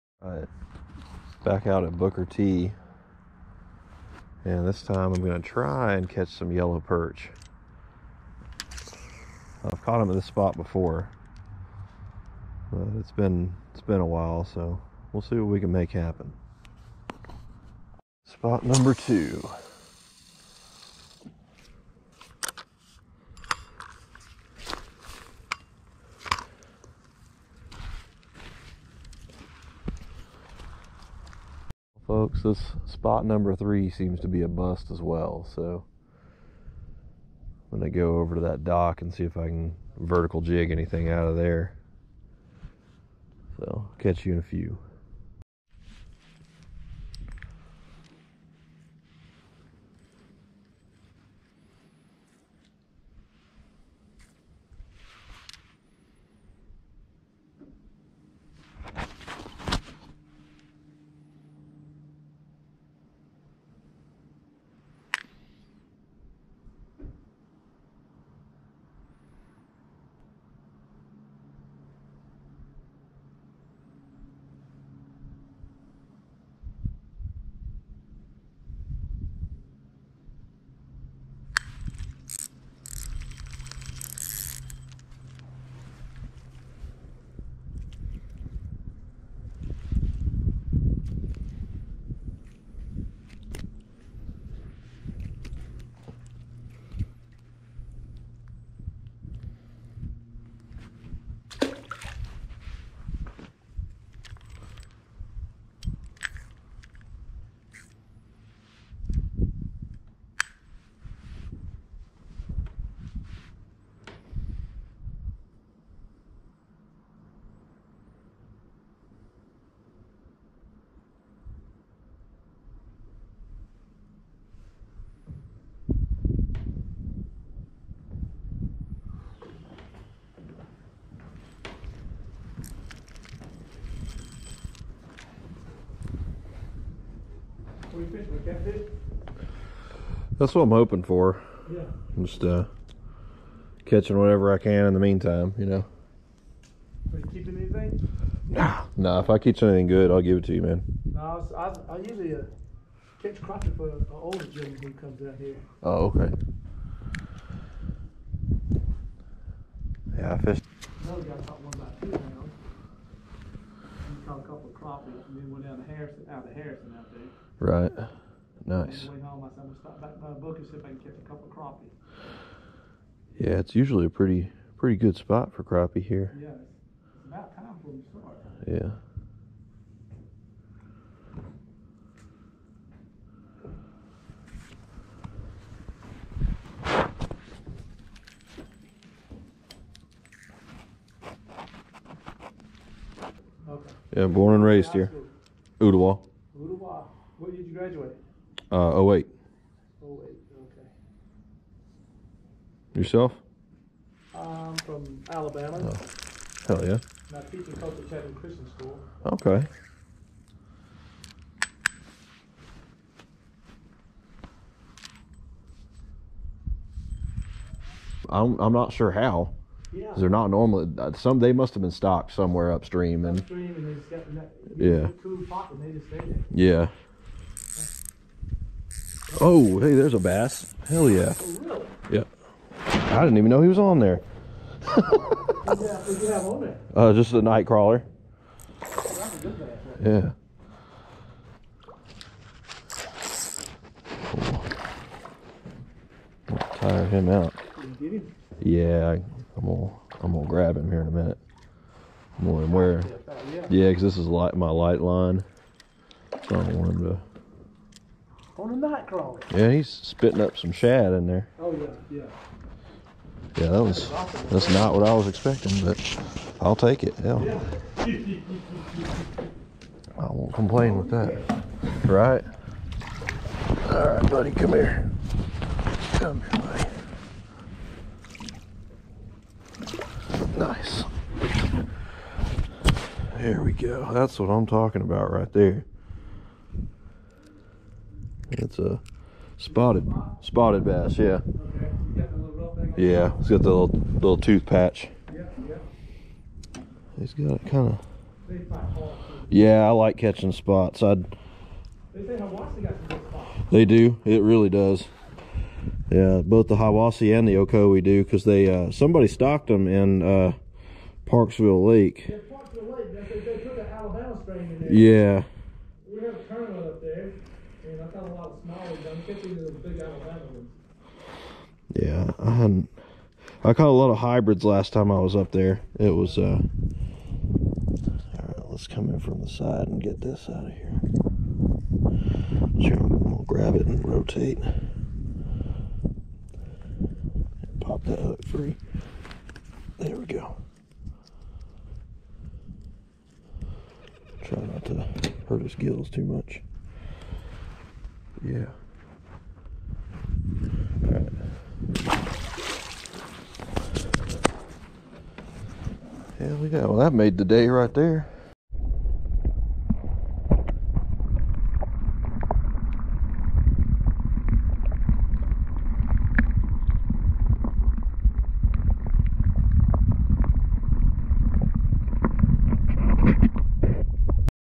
Not the fish I was after but even better.
Didn’t get the perch I was after but the bass more than made up for it. Any fishing day is a good day.